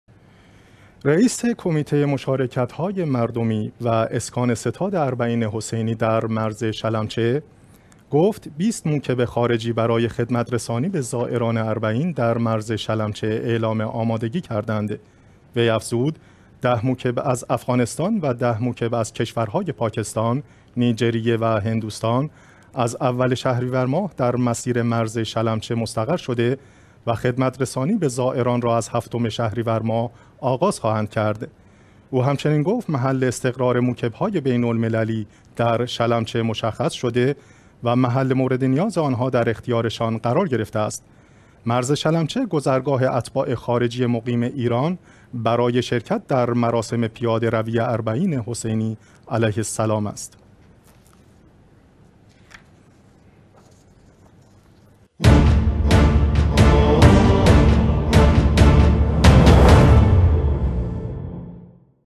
آمادگی ۲۰ موکب غیر ایرانی برای خدمت به زائران اربعین در شلمچه
رئیس کمیته مشارکت‌های مردمی و اسکان ستاد اربعین حسینی در مرز شلمچه گفت: ۲۰ موکب خارجی برای خدمت رسانی به زائران اربعین در مرز شلمچه اعلام آمادگی کردند.
وی افزود: ۱۰ موکب از افغانستان و ۱۰ موکب از کشورهای پاکستان، نیجریه و هندوستان از اول شهریور ماه در مسیر مرز شلمچه مستقر شده و خدمت رسانی به زائران را از هفتم شهریور ماه آغاز خواهند کرد.
او همچنین گفت: محل استقرار موکب‌های بین‌المللی در شلمچه مشخص شده و محل مورد نیاز آنها در اختیارشان قرار گرفته است.
مرز شلمچه، گذرگاه اتباع خارجی مقیم ایران برای شرکت در مراسم پیاده روی اربعین حسینی علیه السلام است.